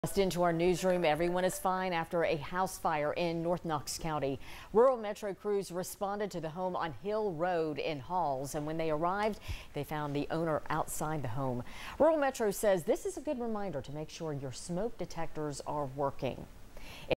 Rural Metro: No injuries reported after house fire in North Knox Co. Tuesday night
Rural Metro said when crews arrived, they found fire on the back side of the home with smoke coming out of it.